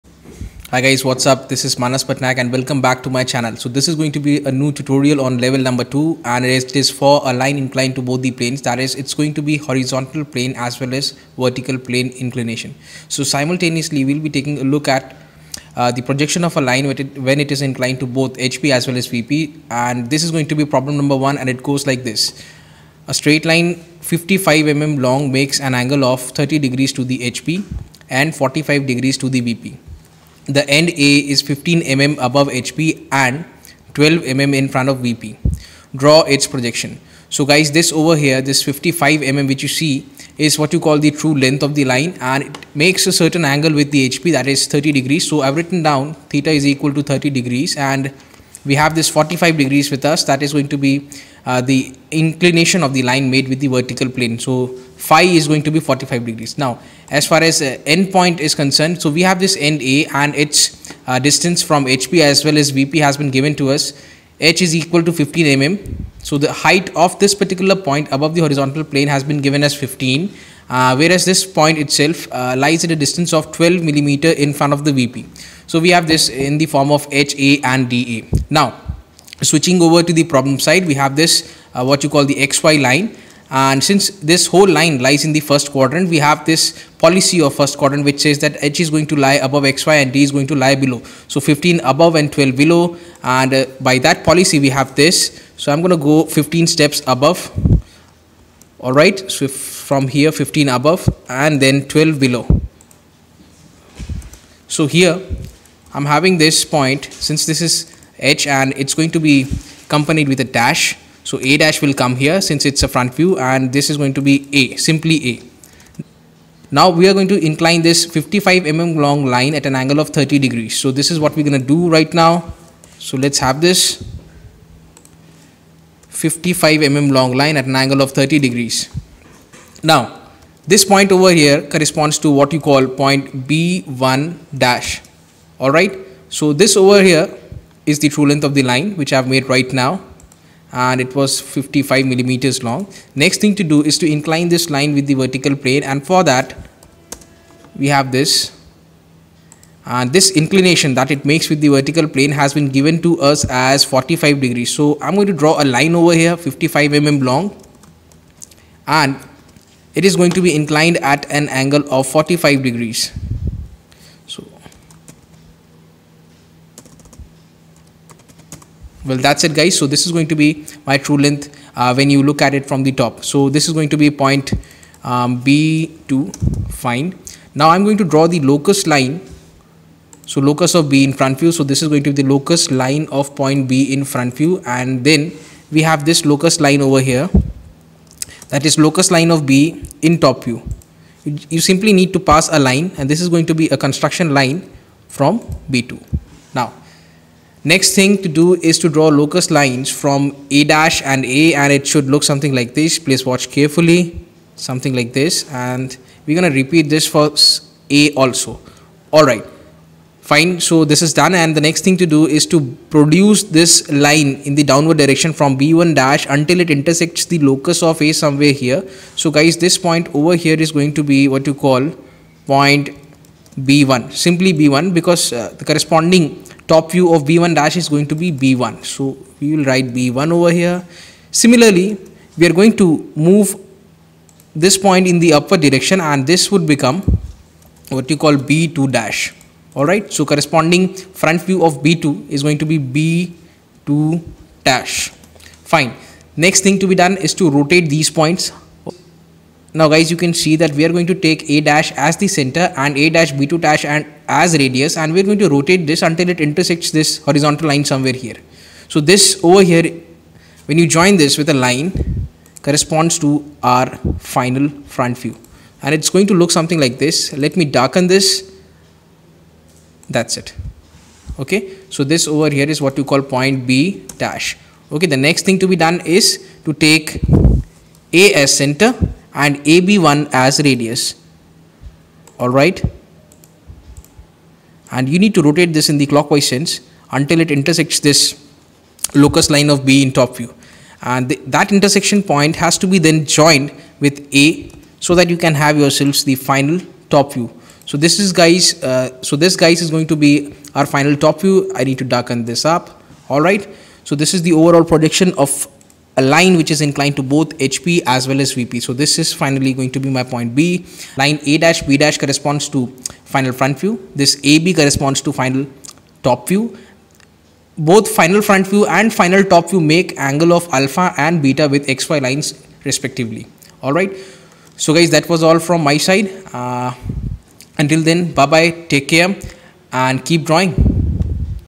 Projection of Lines | Level 2 Problem 1 | Line inclined to both HP and VP
A straight line 55 mm long makes an angle of 30 deg to the HP and 45 deg to the VP. The end A is 12 mm in front of the VP and 15 mm above HP. Draw its projections.